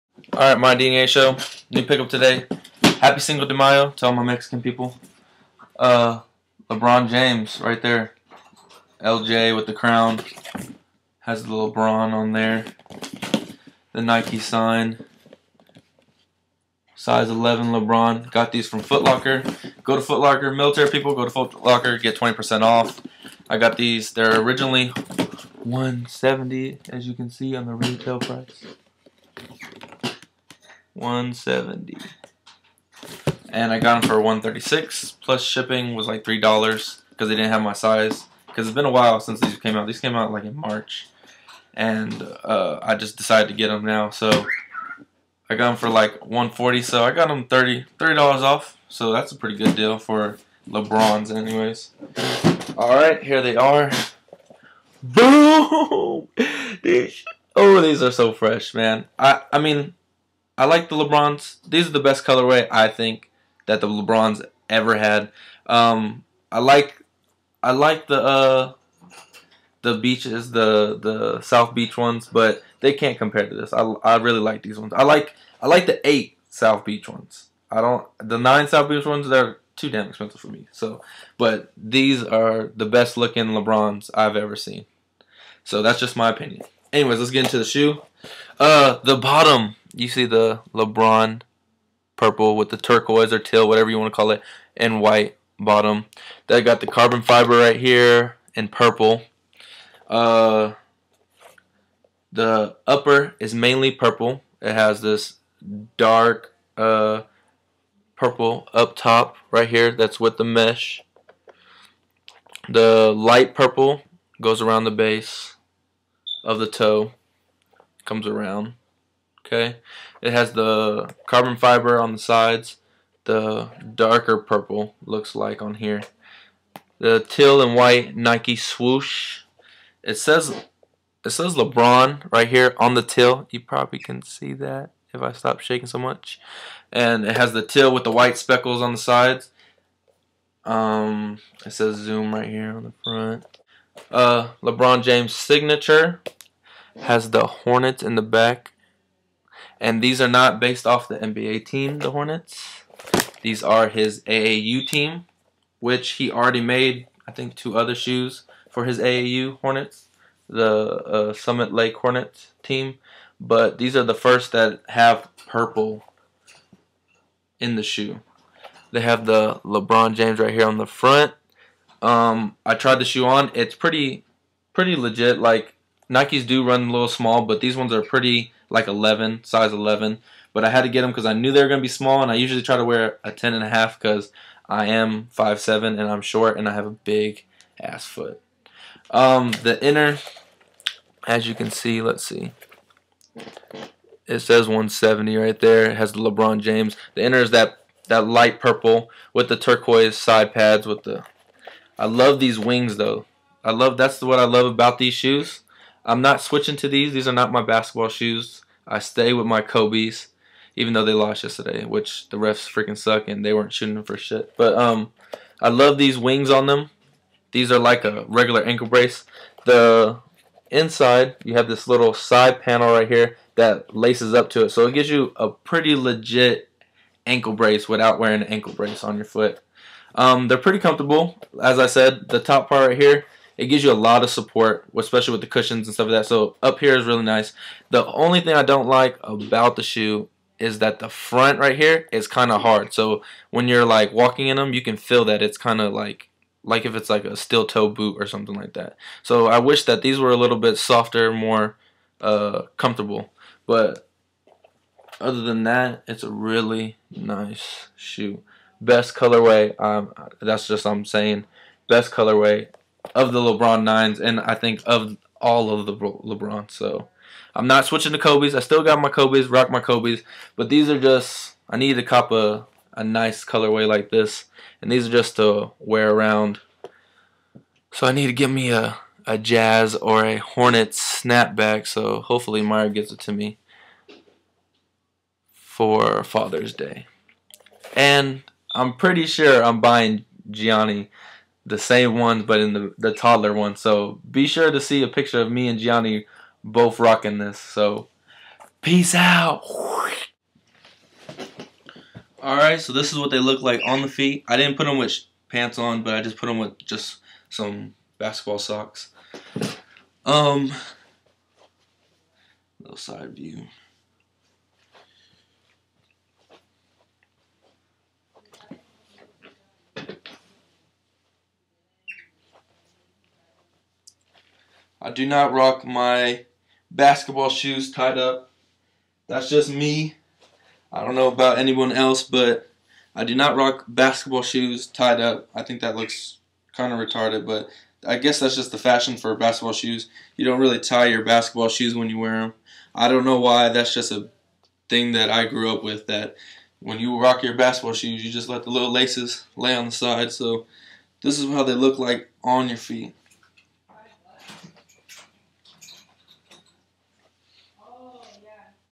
Lebron 9 Summit Lake Hornets
Made in tribute of LeBron's old AAU team, the Summit Lake Hornets. Nike LeBron 9 features a Max Air 180 heel bag and Zoom Air unit in the forefoot of the shoe for maxiumum low-profile cushioning. A carbon based shank gives you enough torsional rigidity and protection so you can play your hardest. Nike LeBron 9 also uses a composite upper design with midfoot wing support and Flywire construction. The signature solid rubber outsole offers durability so you can focus less on your shoes and more on your game.